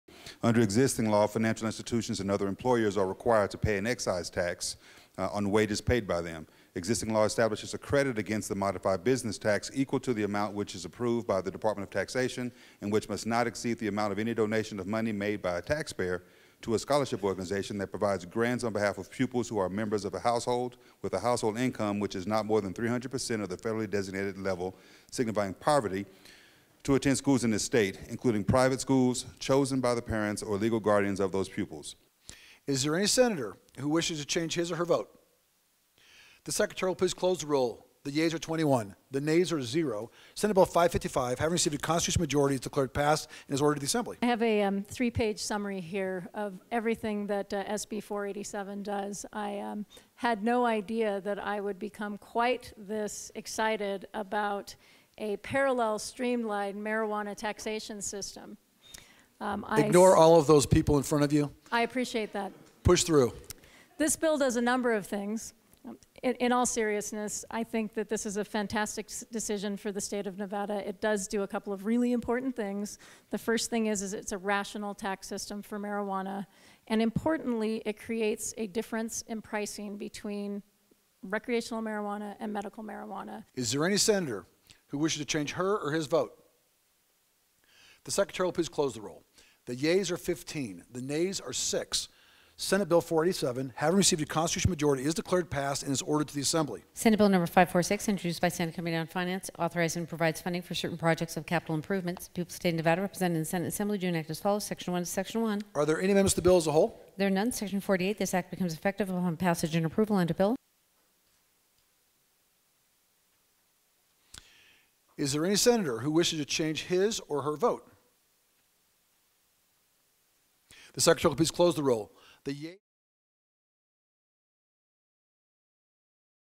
Marathon of State Senate votes kills potential ESA legislation
Three Senate votes within 15 minutes in the waning hours of the legislature effectively killed ESA funding for the legislative session. Elaine Wilson/Las Vegas Review-Journal